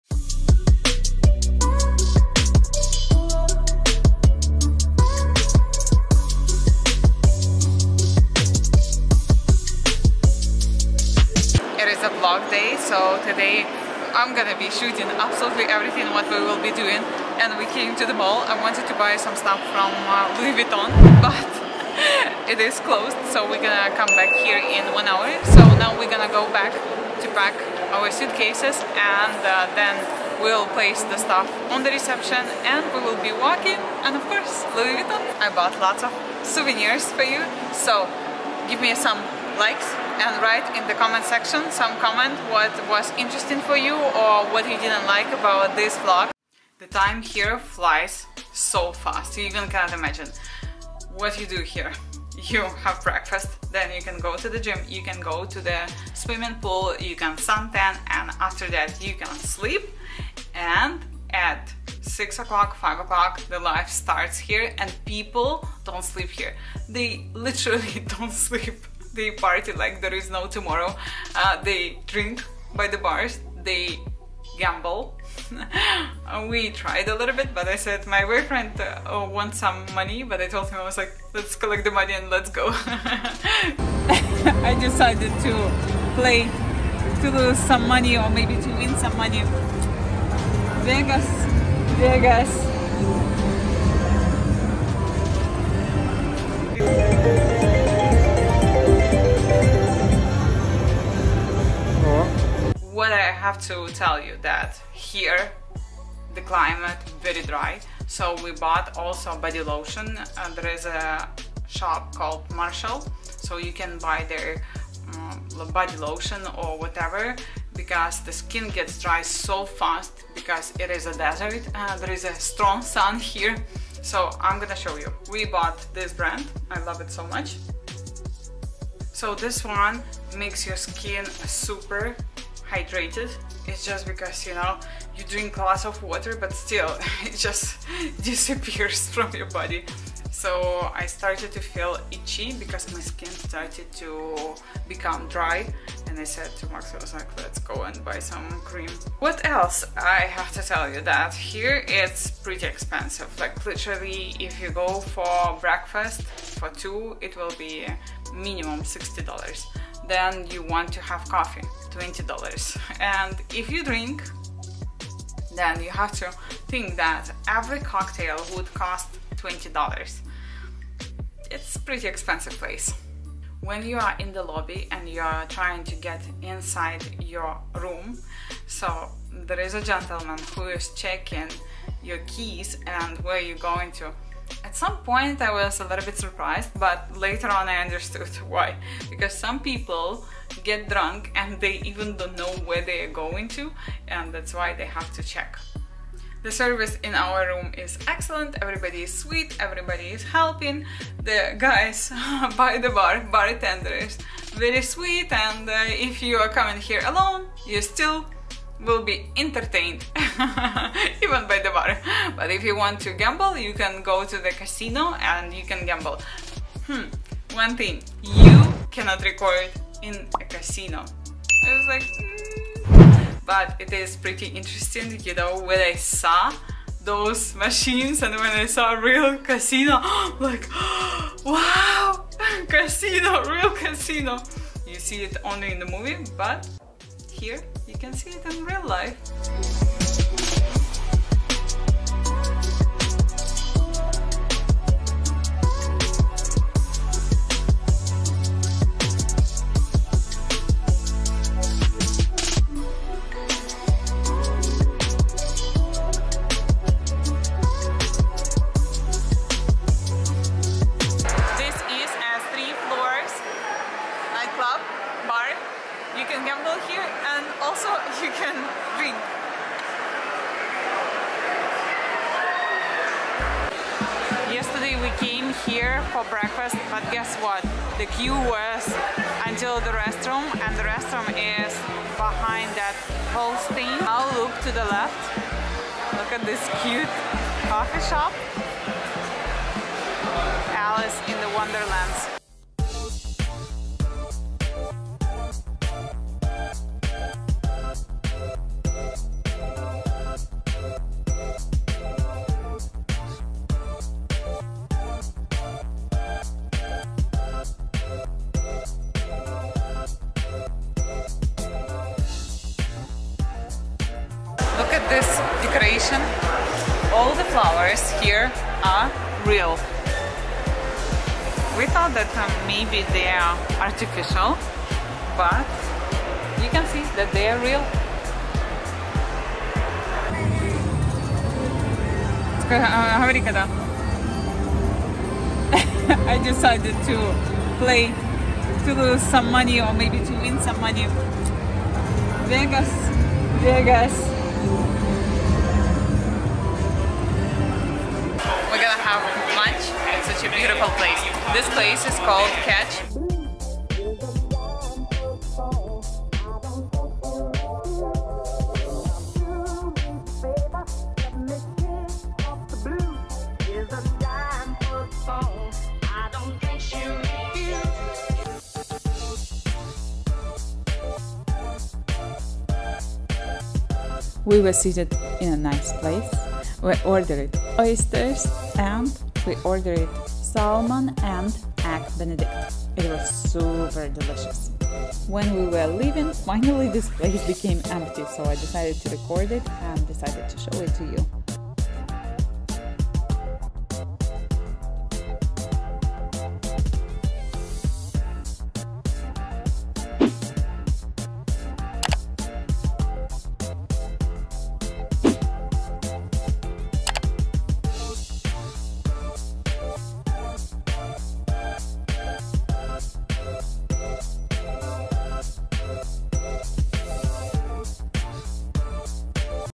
Gambling || cosmopolitan hotel || Catch Las Vegas || Louis Vuitton || Aria hotel
Last day, I woke up so early and could not fall asleep. I decided to record the view from our hotel windows.
At 9am we went to Louis Vuitton shop, but they said that they are closed, despite on Google was written that they were open from midnight. MAke sense - you get drunk and go buy something in LV, or, you win money in a casino and you grab all the money and start spending it straight away 😆😆

We returned back to the hotel, we checked out and went to LV again, I bought sneakers there.

After that, we went to a "Catch" restaurant. Tell me, who was not dreaming to get there if all celebrities were there? 🙃 The other day were trying to book a table there for dinner, but it was full booked 3 weeks ahead. We were like 😳😳. But if you want to have brunch - you are more than welcome!

After that we weren't to Cosmopolitan hotel. I like all the decorations there, so I filmed a bit there for you. They have lots of nice bistros.

At 4pm, I had a heat stroke and we called it the end of the day ☹️☹️
I was feeling nauseous. We thought if I don't recover, we will stay one more night in Vegas. But thank God by8pm, I felt much better, and we headed to the airport.